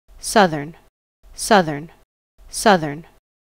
How to Say or Pronounce Southern
This video shows you how to say or pronounce Southern.

How accurate does it say Southern? How would you say Southern?